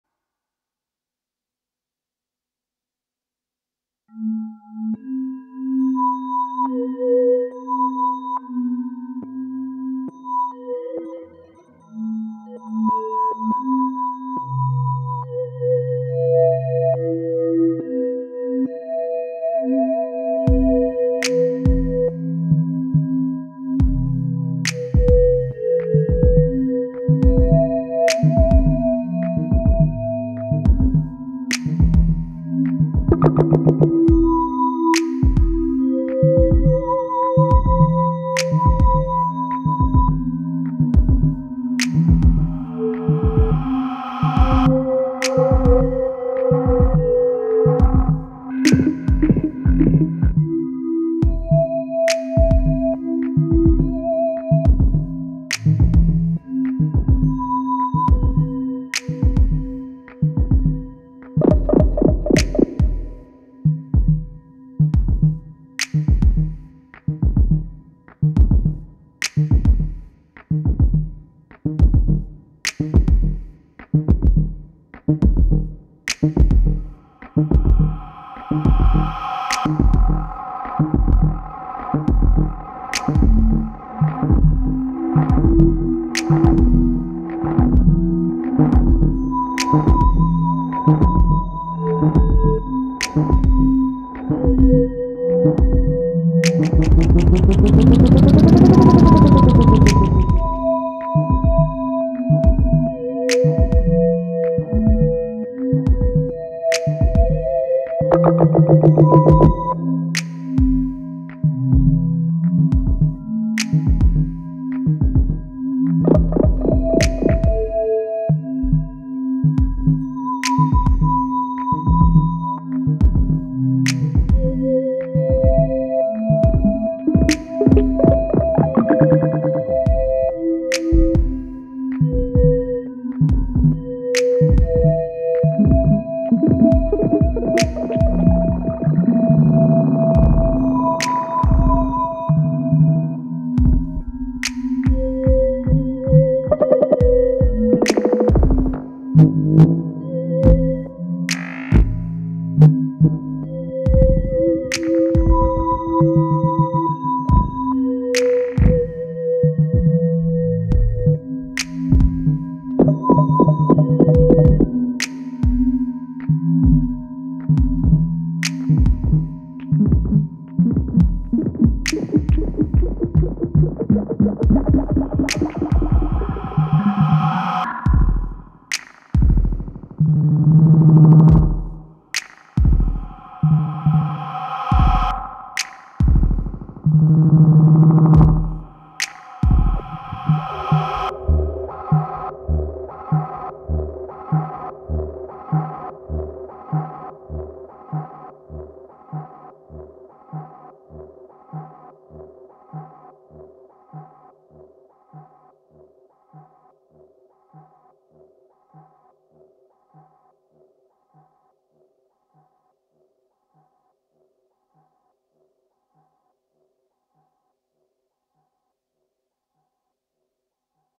Ambient Sketch w/ Korg Volca Sample/FM and Boss DD-500
Going for more of an ambient vibe with this one. I had a simple beat going and put a semi-oscillating analog delay patch on it, then I spent some time playing with the delay subdivisions which is where those bleeps and chirps come from. The FM is playing something random I sequenced, added an arp to, then ran both lines into a Boss RV-5.

I'm also trying uploading this vid in a higher quality format to see how it looks and sounds compared to the smaller format.